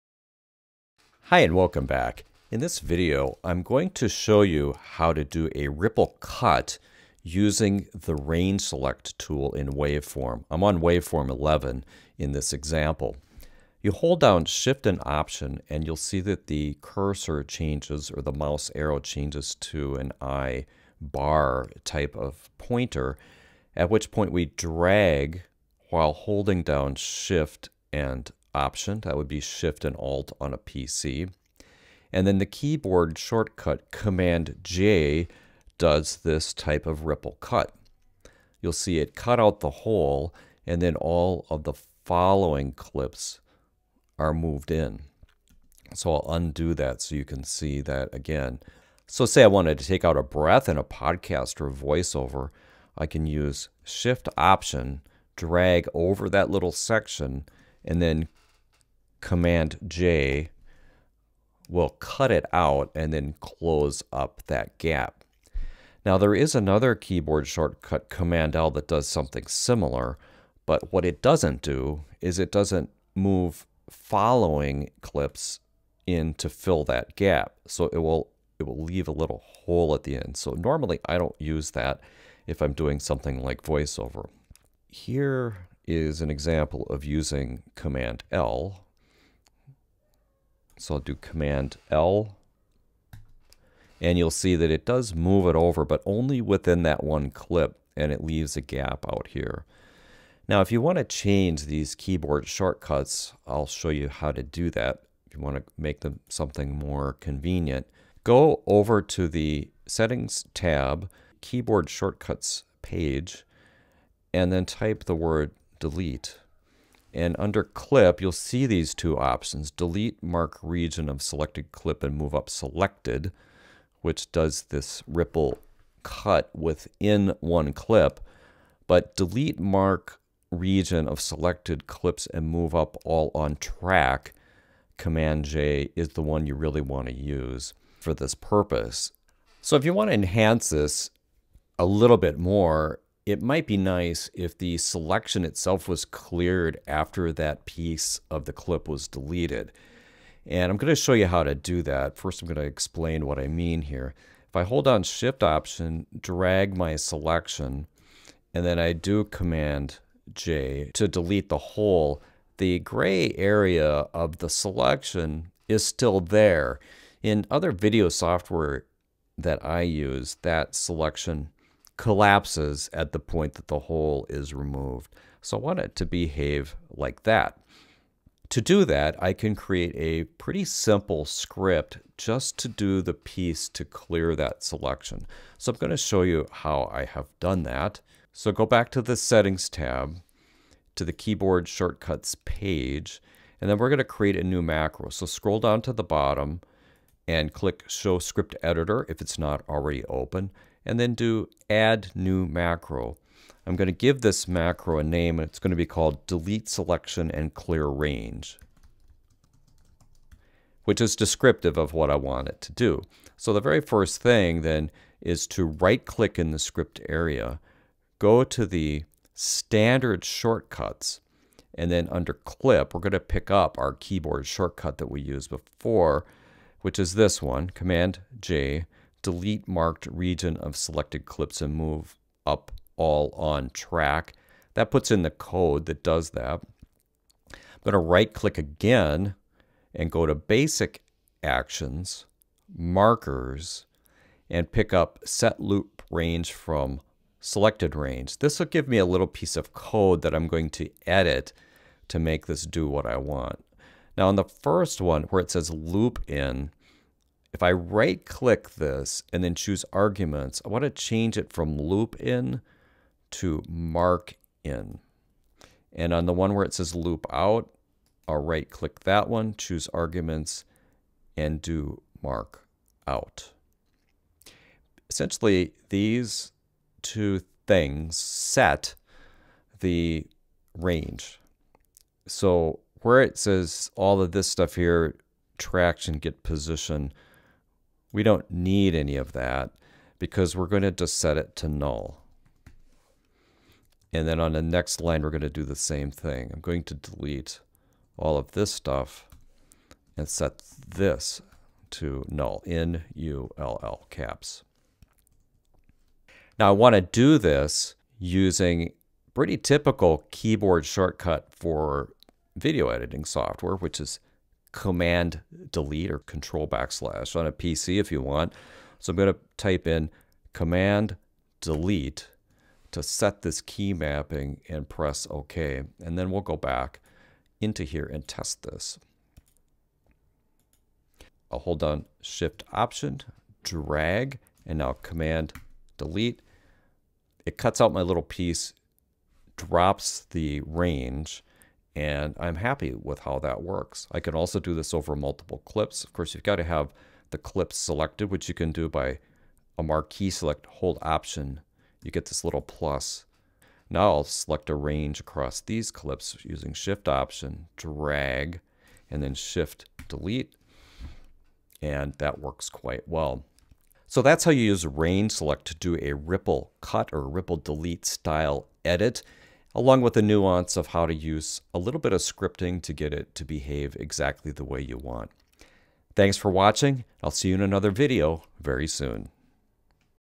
Tracktion Waveform 11: Ripple Delete Range (Video 3)
This video continues the topic of range selection using the Shift + Opt / Shift + Alt cursor introduced with Waveform 10.

The ripple delete is done using the standard Cmd + J / Ctrl + J shortcut following the range selection. That is demonstrated here.

To enhance the ripple delete a bit more, I show how to create a script and assign it to Cmd + Backspace which deletes the selection then clears the range.